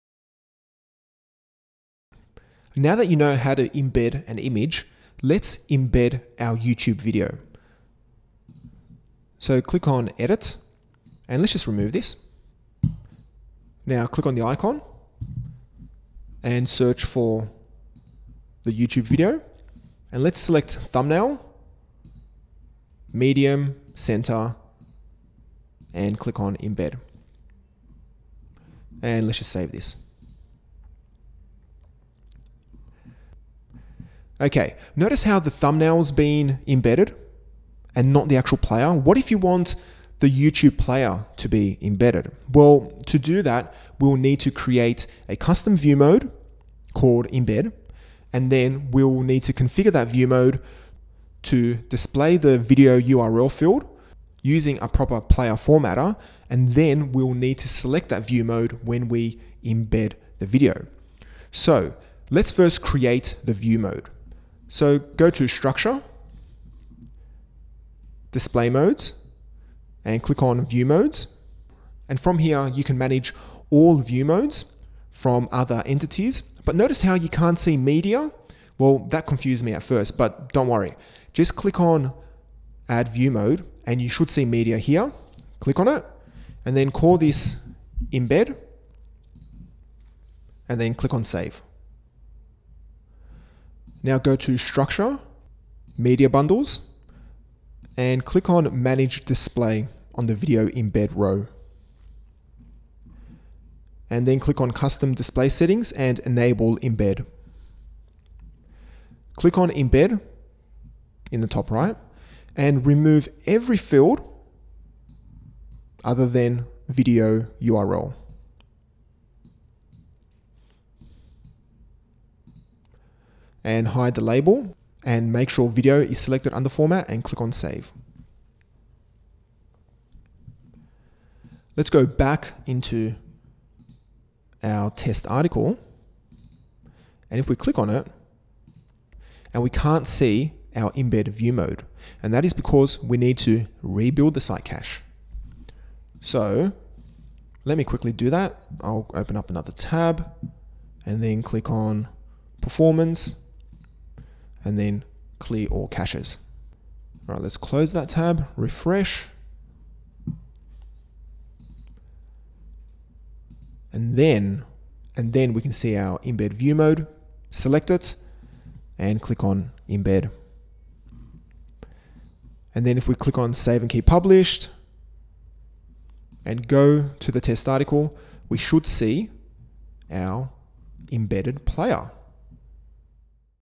Drupal 8 Site Building, 11.4: Handling Embedded Videos
In this video, learn how to display embedded videos by creating a custom view mode.

Sections:
- Create custom view mode (01:08)
- Add view mode Video Embed bundle (01:41)
- Choose custom view mode from Entity Embed pop-up (02:44)